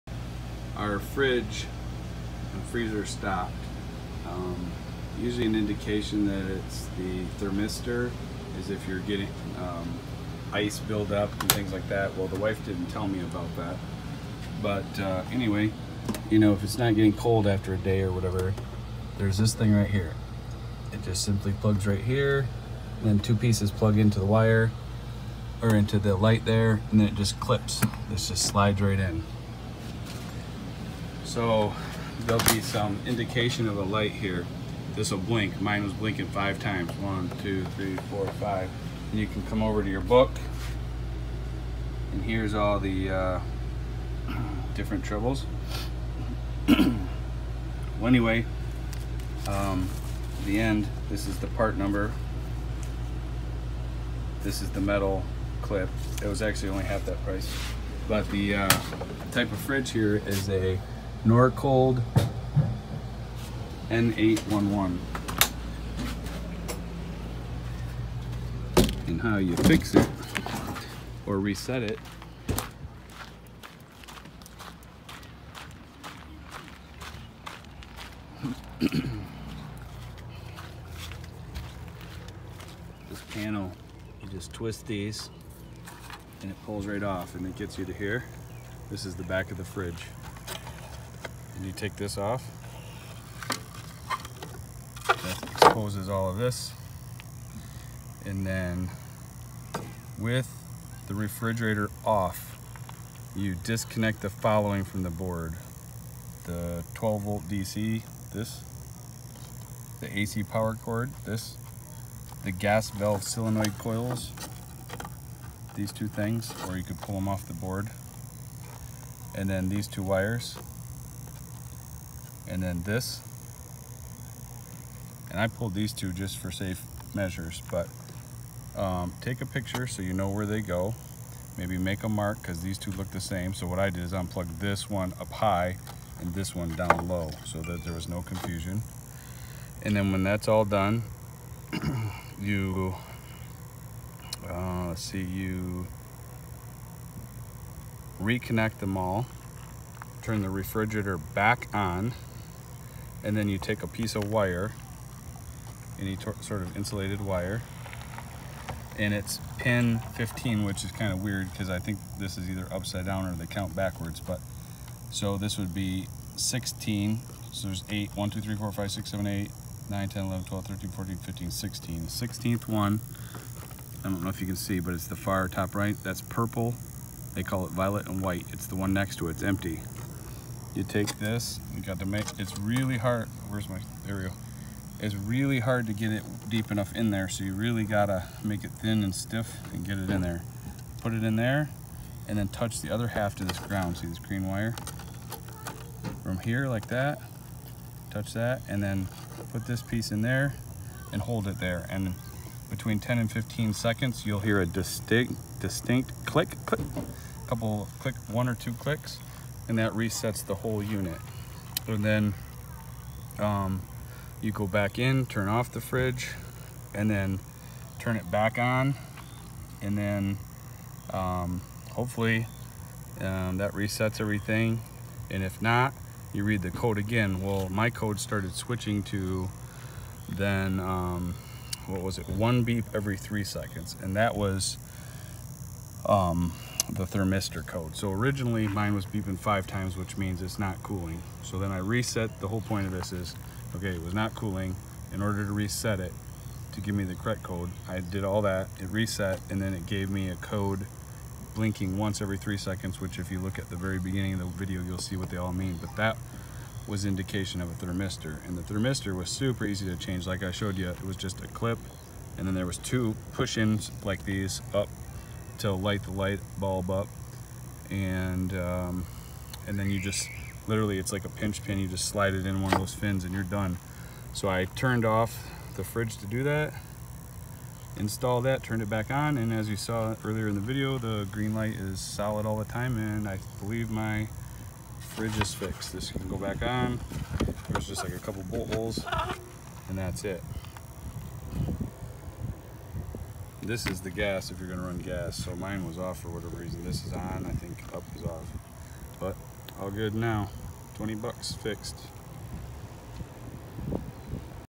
Diagnose and repair Norcold N811 RV camper refrigerator / freezer Jayco
This walks you through exactly how to diagnose and repair your RV refrigerator